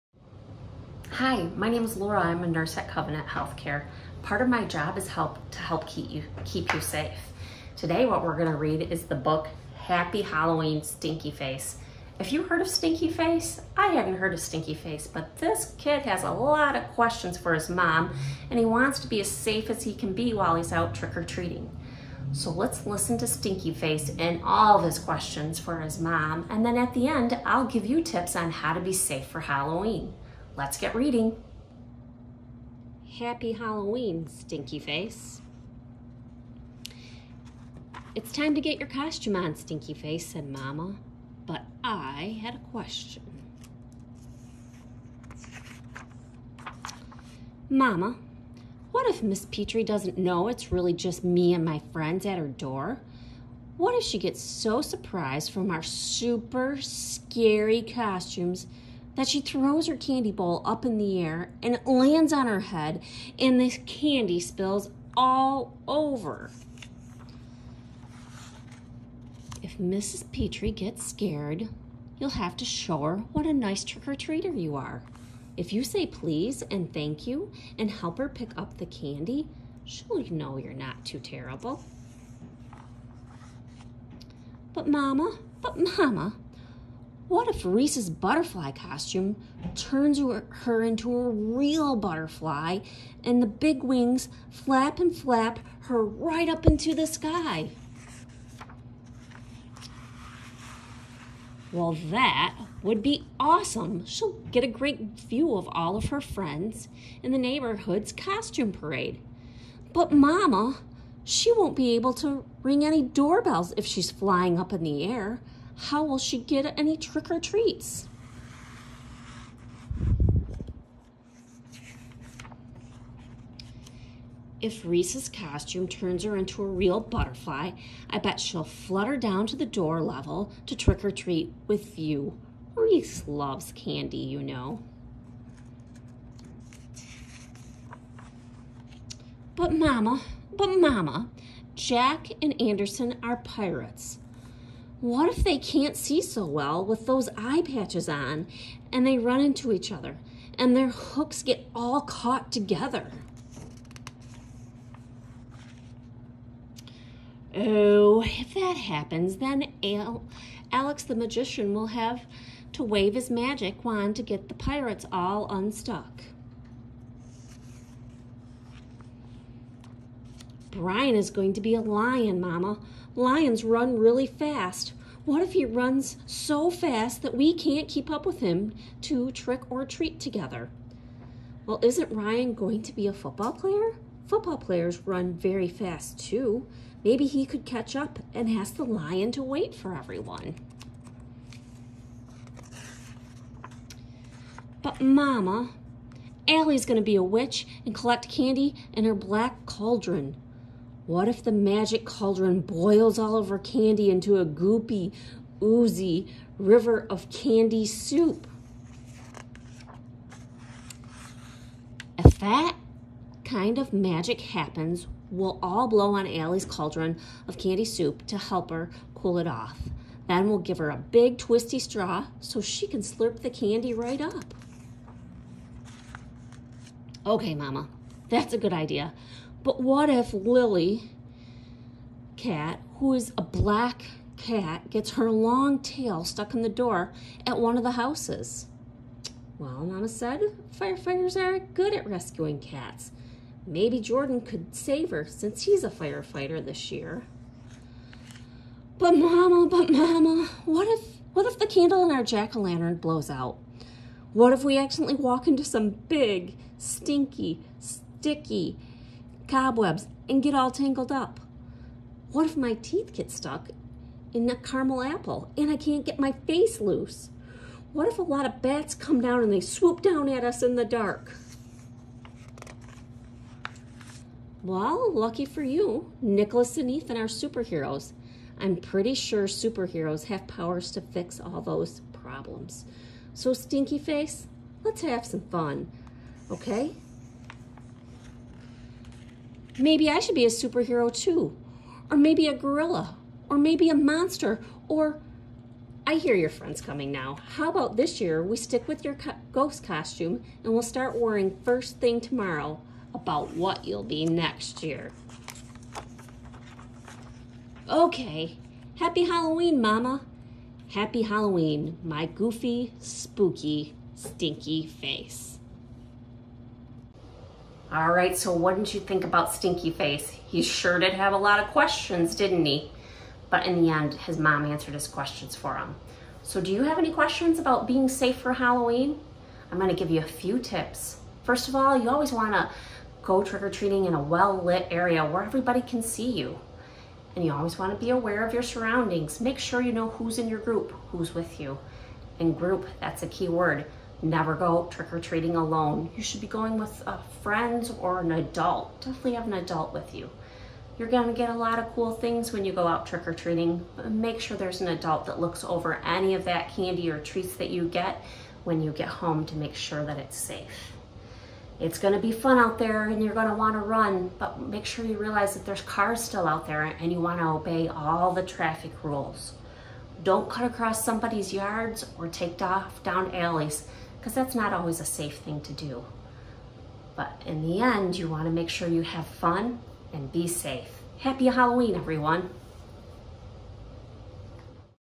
Covenant Story Time: Happy Halloween Stinky Face
Laura Walker, RN, EMT, CFRN with the Covenant Injury Prevention Program, hosts a virtual storytime with "Happy Halloween Stinky Face" and shares tips for safe trick or treating.